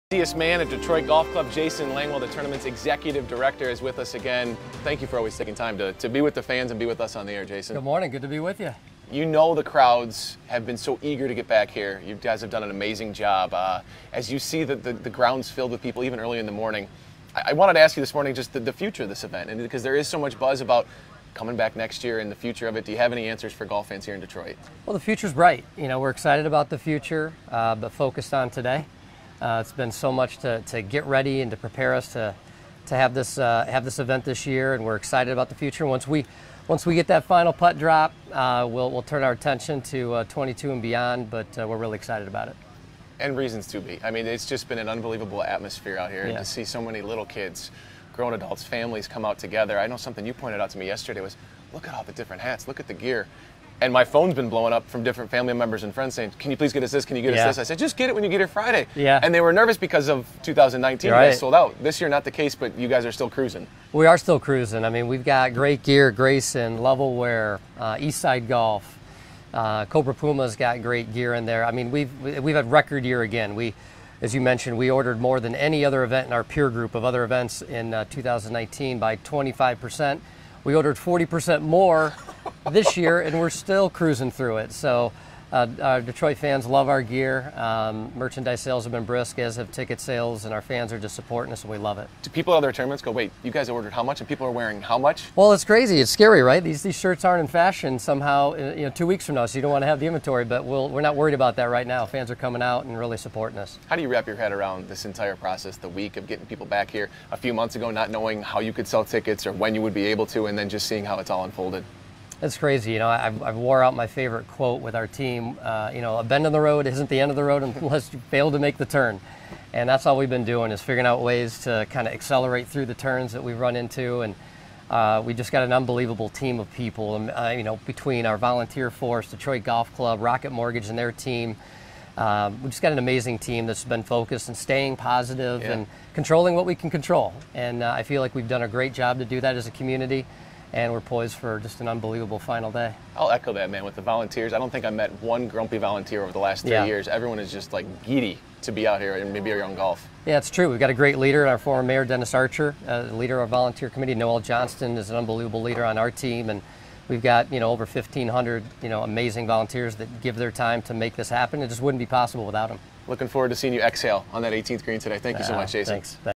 Future of the Rocket Mortgage Classic is 'bright,' tournament director says
"The future is bright" with the Rocket Mortgage Classic, according to tournament director Jason Langwell.